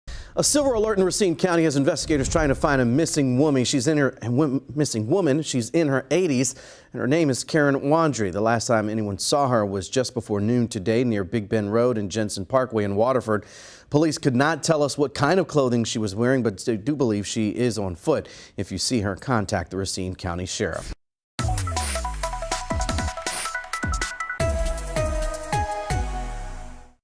Silver Alert issued for missing 85-year-old Racine County woman
Police are searching for a missing 85-year-old woman who was last seen just before noon Tuesday near Big Bend Road and Jensen Parkway in Waterford.